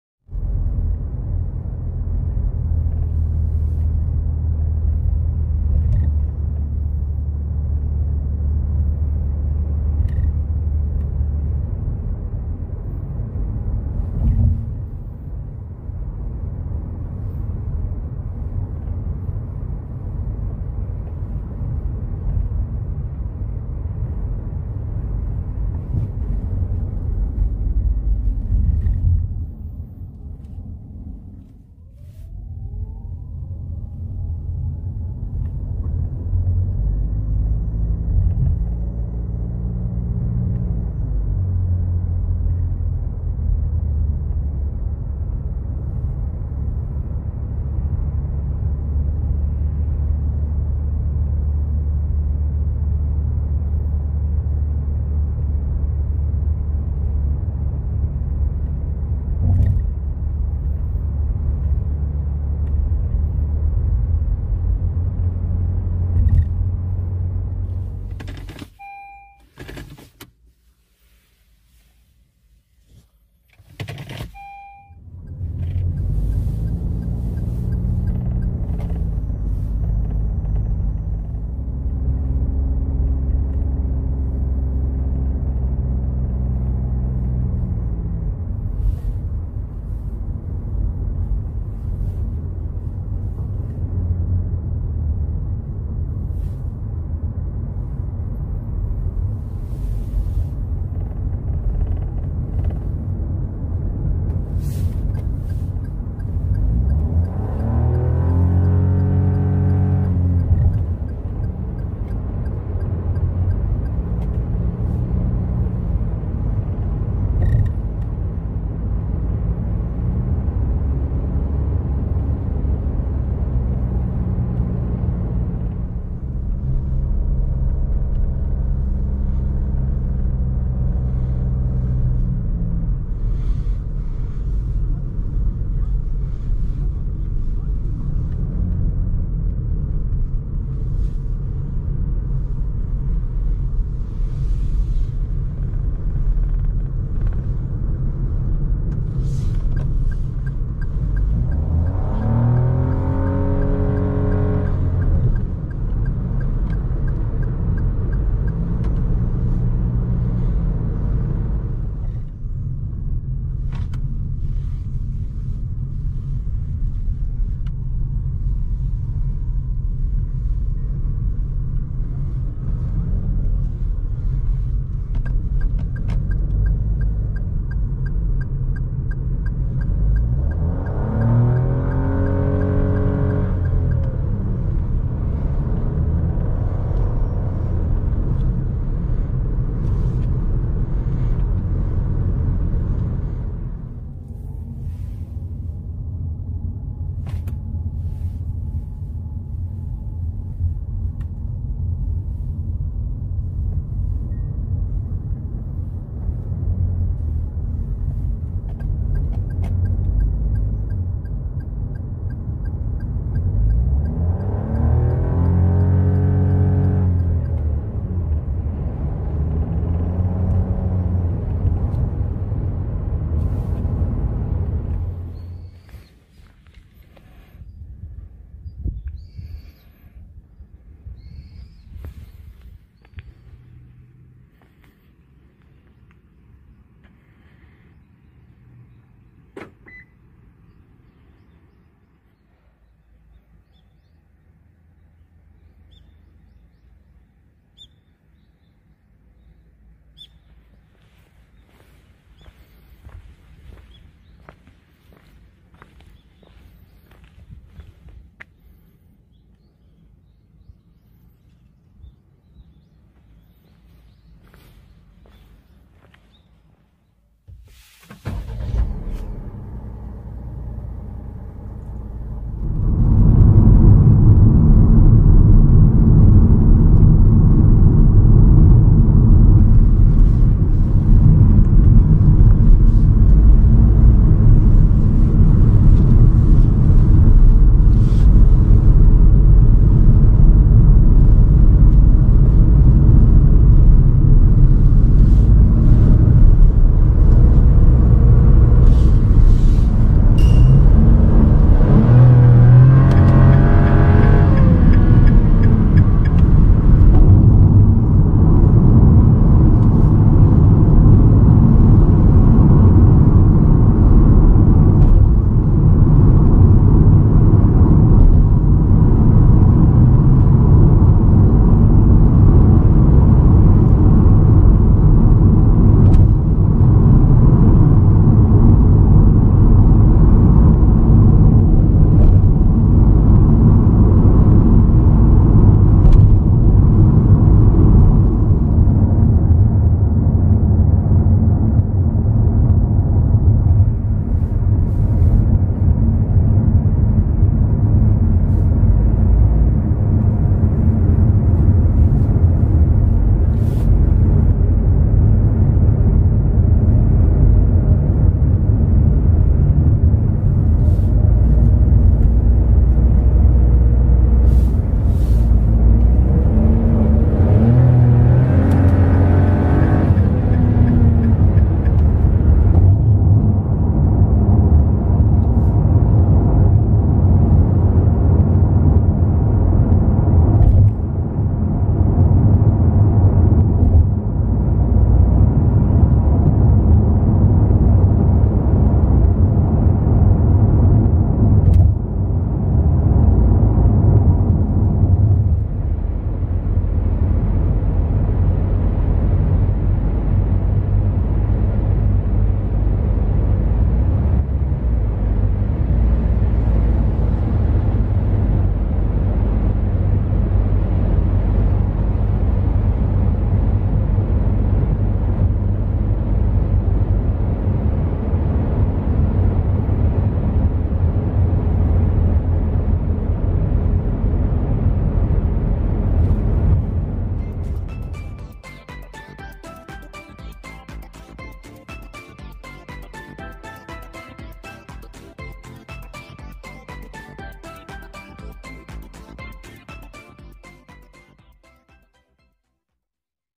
新型ライズ 1.0L ターボ vs ロッキー 1.2L ハイブリッド！ベタ踏みして加速力を比較した結果！
➡︎コンパクトSUVの人気車のダイハツ「ロッキーハイブリッド」OEM車のトヨタ「ライズ ターボ」
1.2Lハイブリッド、1.0Lターボ、 どちらが速いか比較してみた。